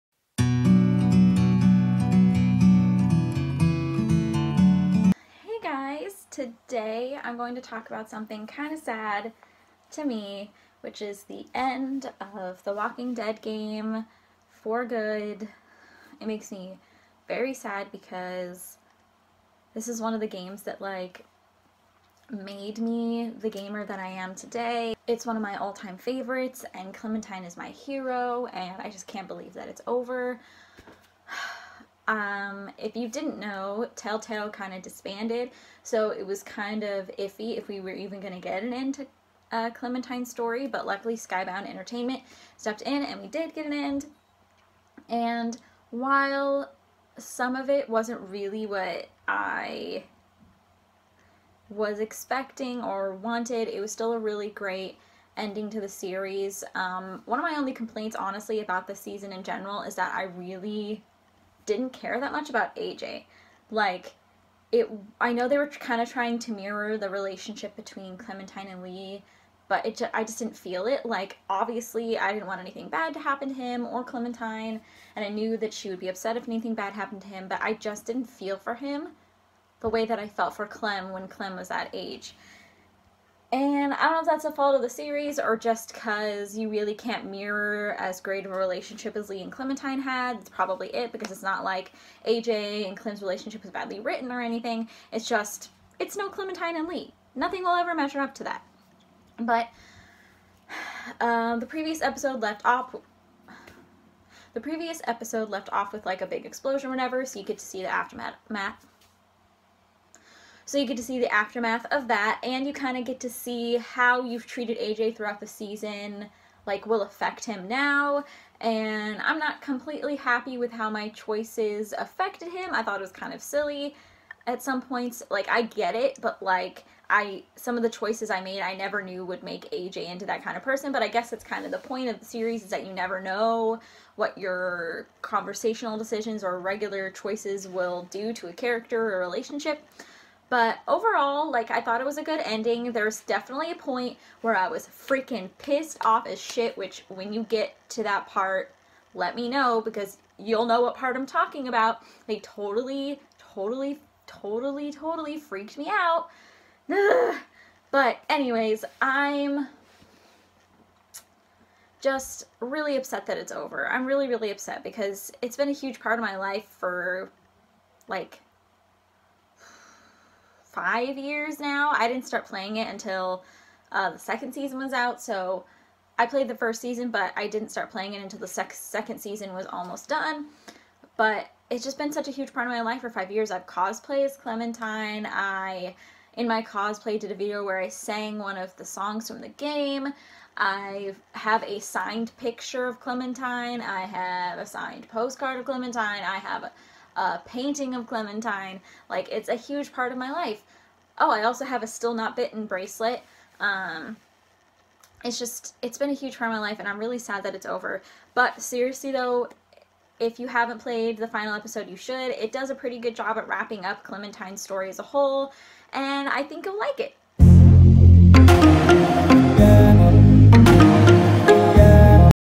Take Us Back: Walking Dead Ending Review
Today I discuss the ending to one of my favorite video games of all time, The Walking Dead game! :(